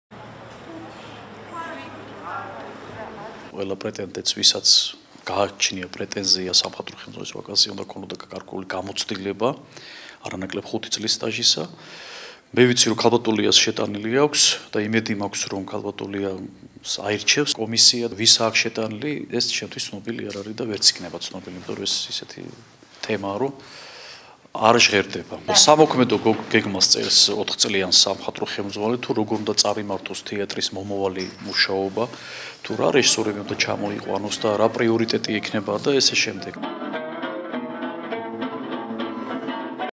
კულტურის სამინისტრო მესხეთის თეატრის სამხატვრო ხელმძღვანელს არჩევს
კულტურის სამინისტრო მესხეთის სახელმწიფო დრამატული თეატრის სამხატვრო ხელმძღვანელს არჩევს.

ვაკანტურ პოზიციაზე საბუთების შეტანის ვადა 12 იანვარს ამოიწურა. თეატრის ადმინისტრაციაში ამბობენ, რომ კონკურსში ლია სულუაშვილი ისევ მონაწილეობს, რომელიც სამხატვრო ხელმძღვანელად ოთხი წლის წინ დაინიშნა.

კიდევ ვის უნდა ვაკანტური პოზიციის დაკავება, უცნობია. კანდიდატმა უნდა წარადგინოს ოთხწლიანი სამოქმედო გეგმა, მათ ჟიური განიხილავს და კულტურის მინისტრს დასამტკიცებლად წარუდგენს.

მესხეთის სახელმწიფო დრამატული თეატრის სამხატვრო ხელმძღვანელი სავარაუდოდ, იანვრის ბოლოს დაინიშნება. მანამდე მის მოვალეობას დირექტორი, ირაკლი ათუნაშვილი ასრულებს.

ოთხწლიანი ვადა ამოეწურა ახალციხის თოჯინების თეატრის სამხატვრო ხელმძღვანელსაც. ვახტანგ ქორიძე ამბობს, რომ პოზიციაზე კონკურსი კულტურის სამინისტროს ჯერ არ გამოუცხადებია.